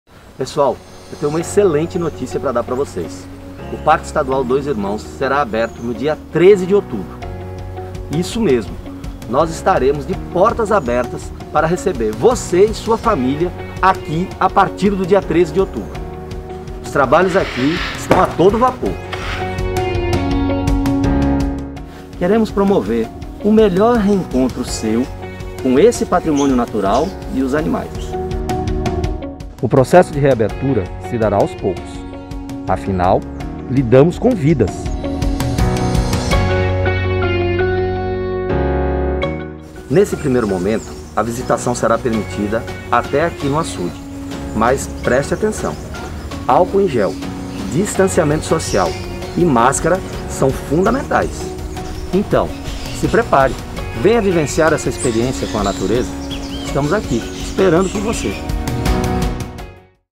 Parque Estadual de Dois Irmãos reabrirá ao público a partir de 13 de outubro
O Parque Estadual de Dois Irmãos já tem data certa para voltar a receber visitação pública: dia 13 de outubro. O anúncio foi feito pela Secretaria de Meio Ambiente e Sustentabilidade de Pernambuco (Semas/PE), órgão responsável pela administração do equipamento. Fechado há cerca de um ano meio por causa da pandemia, o espaço – que é formado por uma importante reserva de Mata Atlântica e pelo Zoológico do Recife – poderá receber até 3 mil pessoas por dia, respeitando todos os cuidados para evitar a disseminação da Covid-19. Para isso, o local já passa por uma série de serviços de manutenção, como jardinagem, paisagismo, melhorias estruturais dos recintos e pintura.

“Queremos promover o melhor reencontro possível dos pernambucanos e visitantes com esse patrimônio natural e com as inúmeras espécies que vivem no parque. O trabalho de manutenção está a todo vapor, já tendo sido recuperados 34 recintos. O processo de reabertura se dará aos poucos, permitindo a melhor readaptação dos animais à visitação e garantindo a segurança de todos os envolvidos na operação”, afirmou o secretário de Meio Ambiente e Sustentabilidade, José Bertotti. Além dos serviços de melhorias nos recintos, o parque passa por paisagismo, implantação de novos bancos e reforma dos banheiros.

O parque vai reabrir por etapas, liberando gradativamente as áreas de visitação. Nessa primeira fase, o público poderá percorrer o equipamento desde a entrada até o açude, passeando pelas alamedas onde se encontram as aves, o serpentário, diversos répteis (jacarés, iguanas, etc) e pequenos mamíferos (jaguatirica, tamanduá-bandeira, guaxinim, furão e mais). Além disso, as famílias poderão desfrutar do parque infantil localizado às margens do Açude de Dois Irmãos e de várias áreas de contemplação. Ainda é possível fazer trilhas pela mata conhecendo a exuberância da natureza em uma das mais antigas e preservadas Unidades de Conservação do Estado.

Dentro do processo de reabertura, o equipamento funcionará em horário especial, recebendo o público de quarta a domingo, sempre das 9h às 15h. O visitante precisa ficar atento, pois a venda de ingressos encerrará uma hora antes do fechamento, ou seja, às 14h. Também não haverá funcionamento nos feriados. Quem for ao parque deve levar lanche e água, pois não há comércio dentro do equipamento. É necessário o uso de calças compridas e sapatos fechados para quem opte por fazer trilha pela Mata Atlântica.

Prevenção à Covid – Para acessar as dependências do parque, é obrigatório o uso de máscaras, assim como manter o distanciamento durante o passeio. Placas informativas do protocolo de segurança e totens com álcool em gel estão sendo colocados ao longo do equipamento para reforçar a mensagem sobre os cuidados necessários e evitar a disseminação da Covid-19. Pinturas no chão ainda vão indicar às pessoas o melhor local para se posicionar em frente aos recintos, estimulando o distanciamento seguro entre os grupos de visitantes e deles com os animais.

SERVIÇO

Parque Estadual de Dois Irmãos
Reabertura: a partir de 13 de outubro
Funcionamento: de quarta a domingo, das 9h às 14h, exceto feriados.
Ingressos:
Entrada inteira – R$ 5,00 | Meia-entrada: R$ 2,50 | Trilha (agendada) – R$ 5,00